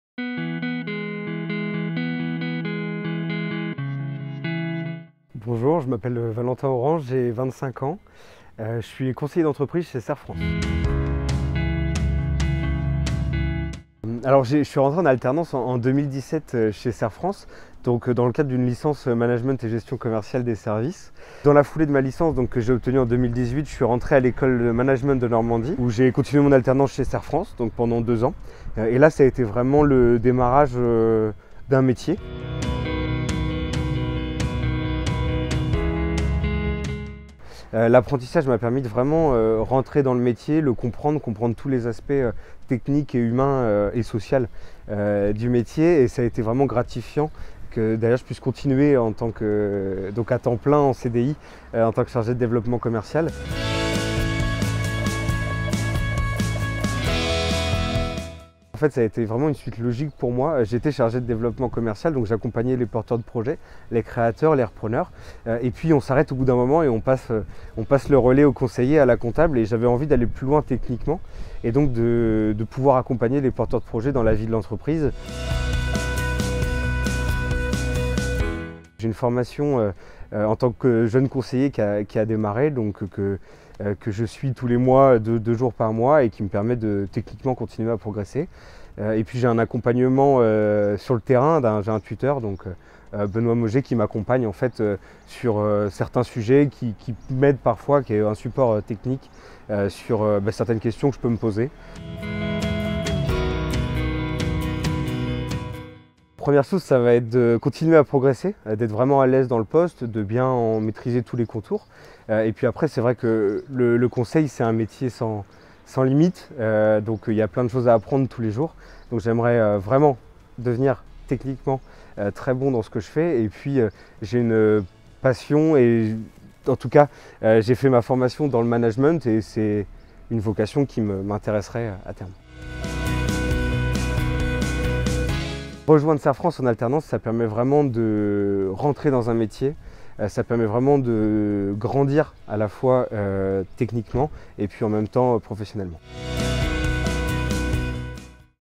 Portrait de collaborateur | Valentin Orange, conseiller d'entreprise chez Cerfrance Seine Normandie
Alternant chez Cerfrance Seine Normandie dès 2017, avant de devenir chargé de développement et aujourd'hui, conseiller d'entreprise, nous sommes fiers du parcours professionnel de Valentin.

L'évolution de carrière de nos collaborateurs au sein de notre entreprise est un élément essentiel 👊
Notre volonté est de créer des conditions optimales pour permettre à chacun d'exprimer au mieux son potentiel.